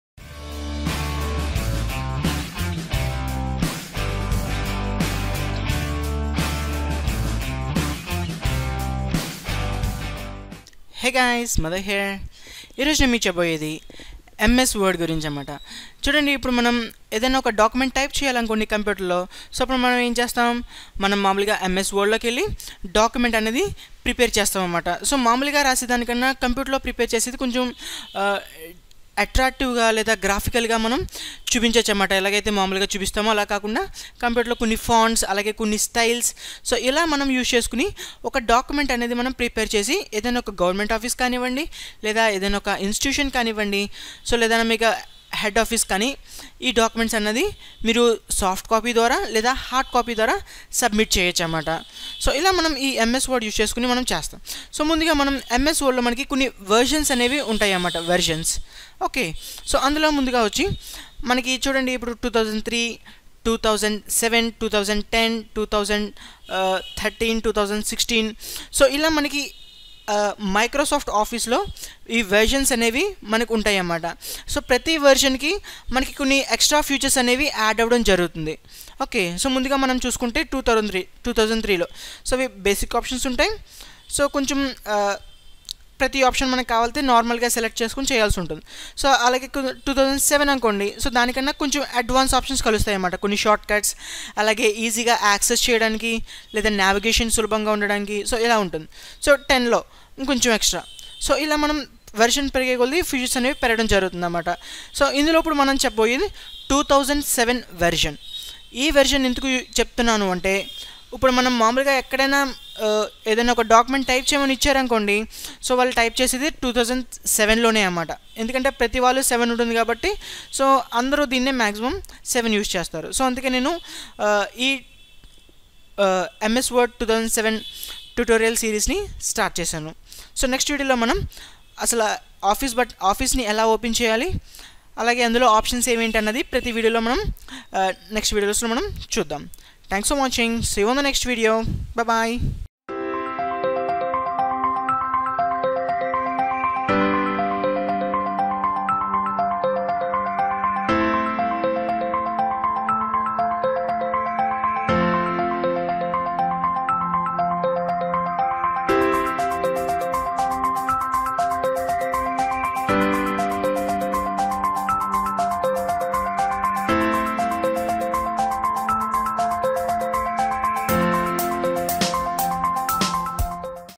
Introduction of Microsoft Office Word 2007
MS Word is a popular application that helps to create a document as you need. By this Telugu tutorial, you can learn MS Word 2007 with complete Instructions